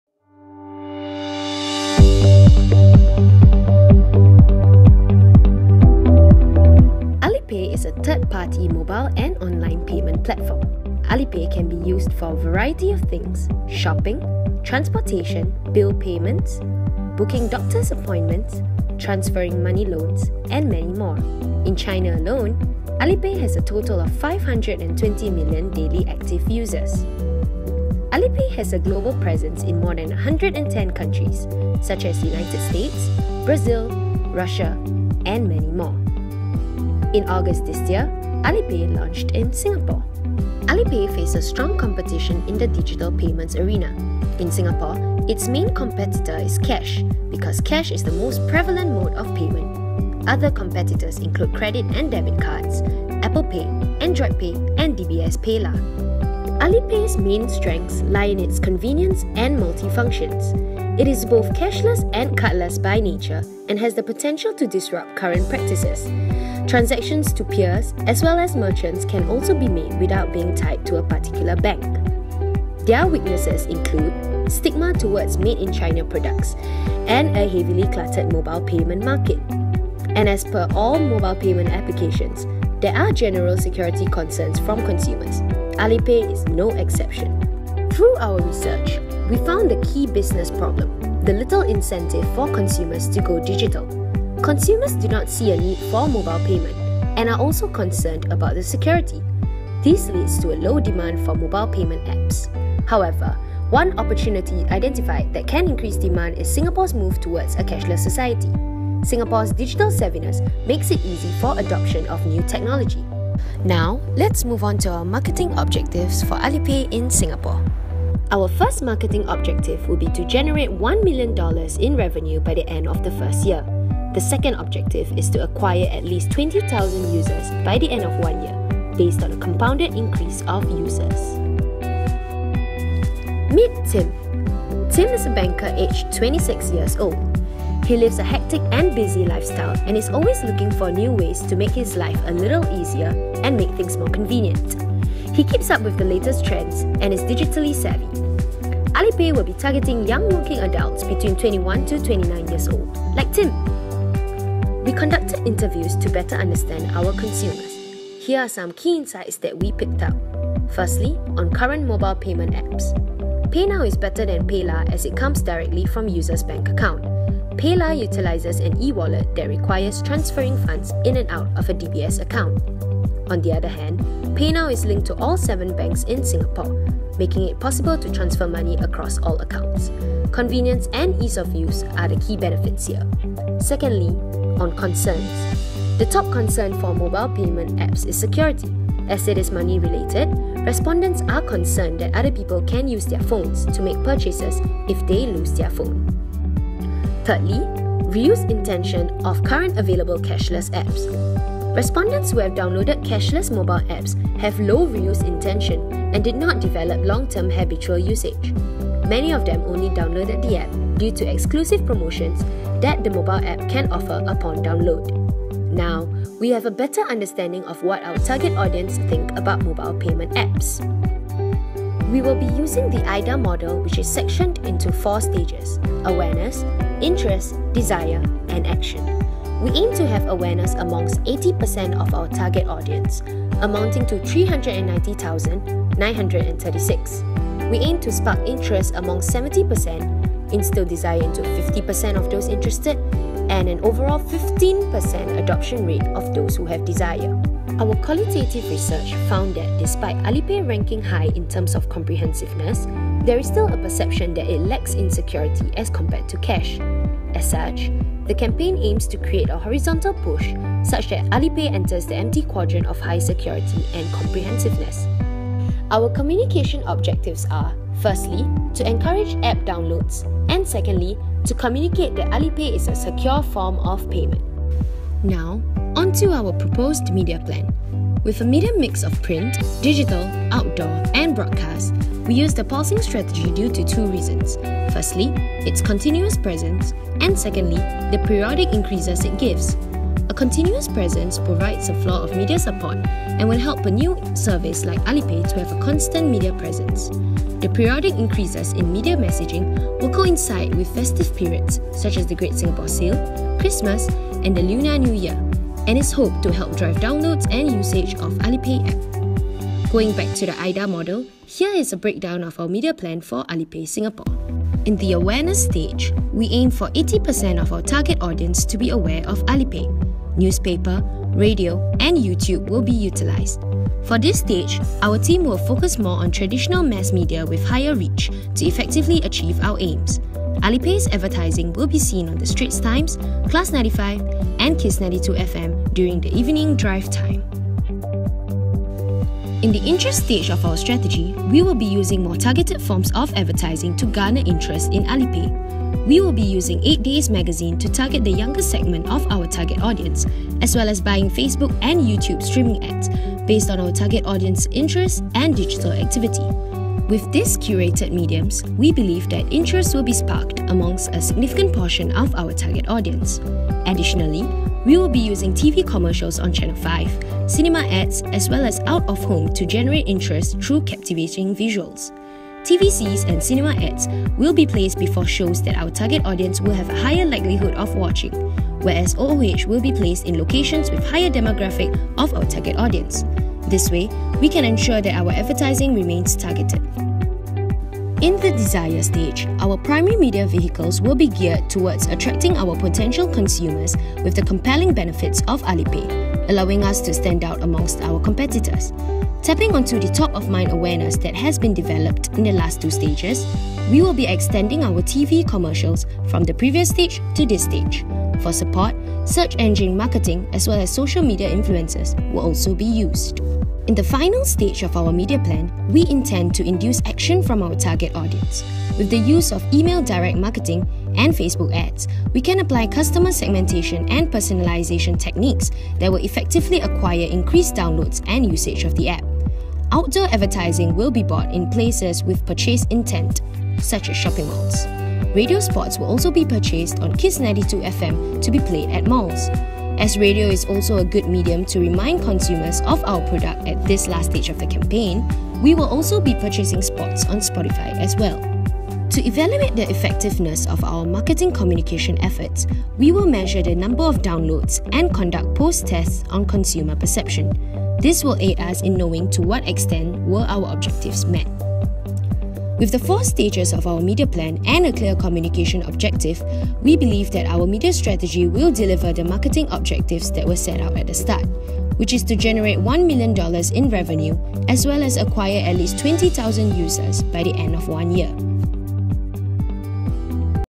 CS4031 ALIPAY
A brand analysis and media plan proposal for Alipay in Singapore.